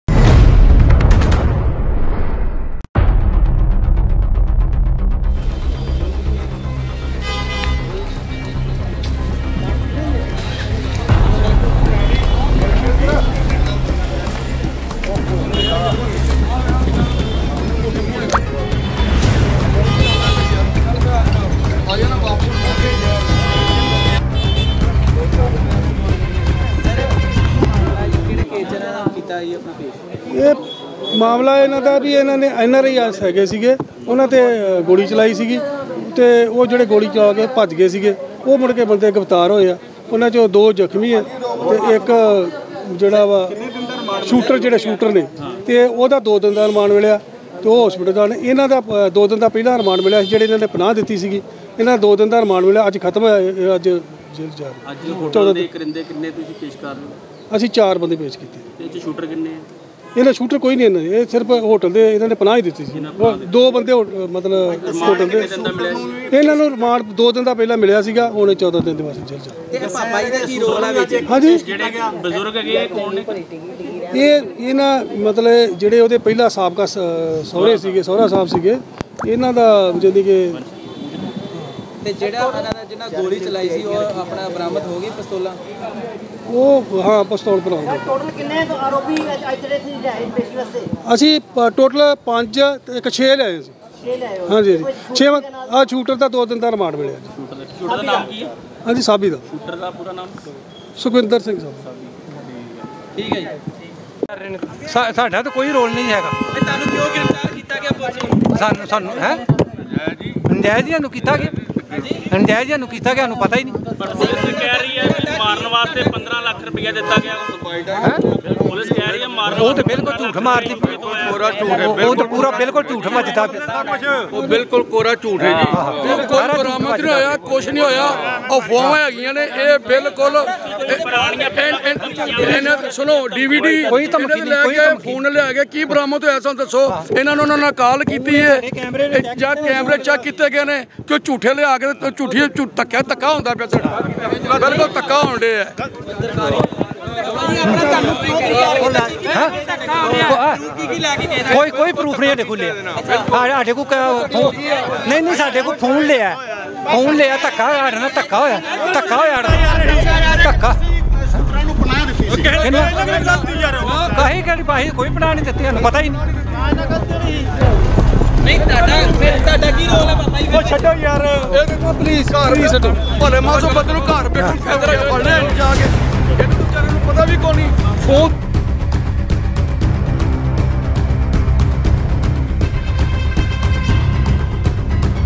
Amritsar NRI Firing accused in court | amritsar NRI sukhchain firing case |nri firing accused remand
Amritsar NRI Firing accused in court | mritsar NRI sukhchain firing case | ri firing accused remand

Today video NRI accused appear in amritsar court,
video today amritsar NRI FIRING accused on police remand ,
firing on nri on daburji accused appear in amritsar court ,

instgrame -@5aabtoday_official